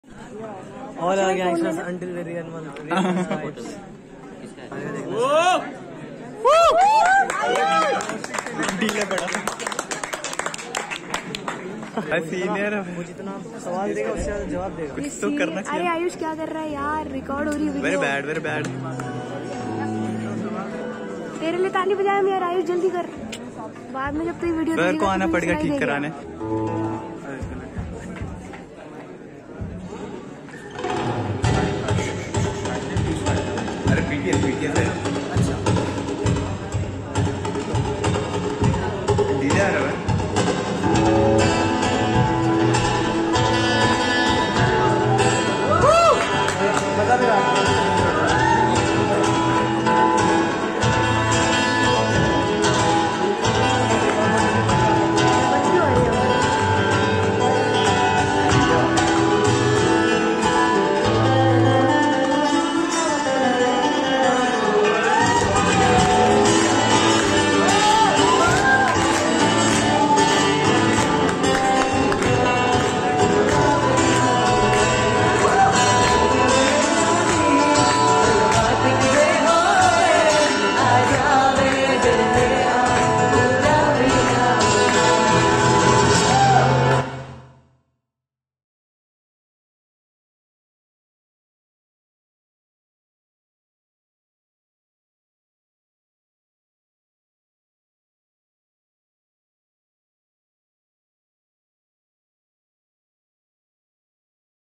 Unexpectedly Audience Started Singing with Fingerstyle Guitar 😳
Performed at @AAFTSCHOOL and Made Audience Sing With My Guitar

Guitar : Kepma A1Ce TransAcoustic
All thanks to @procraftindia For having me this Great choice